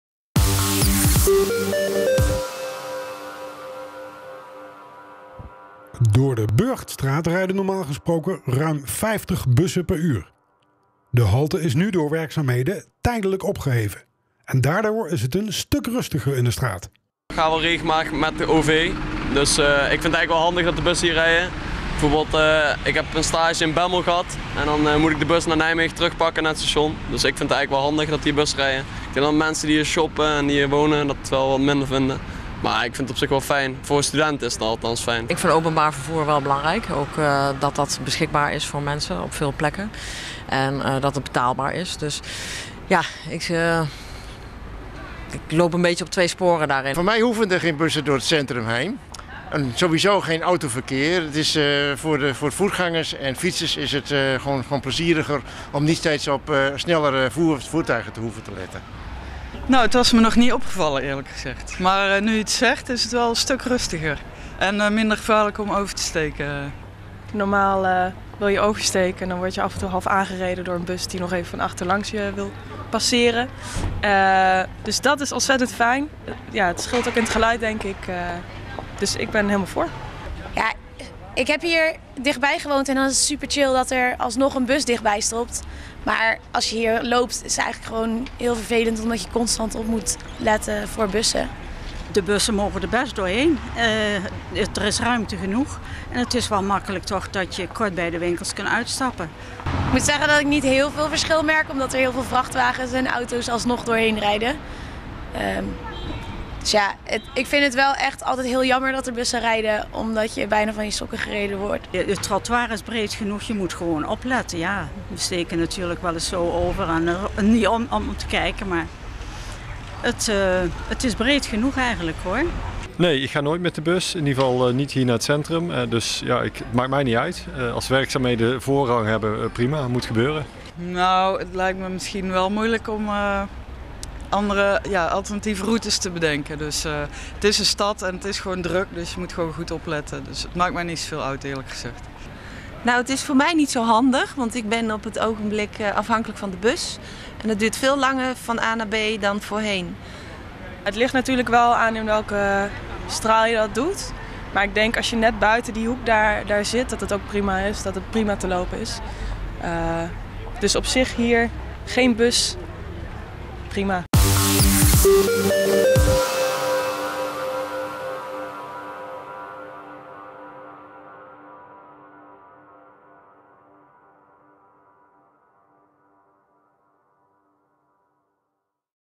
Afwezigheid bus zorgt voor rustige Burchtstraat
De Burchtstraat is tot en met medio juli busvrij. De halte in de straat is door werkzaamheden tijdelijk opgeheven en daardoor is het een stuk rustiger in de straat. Normaal gesproken rijden er ruim 50 bussen per uur langs de halte.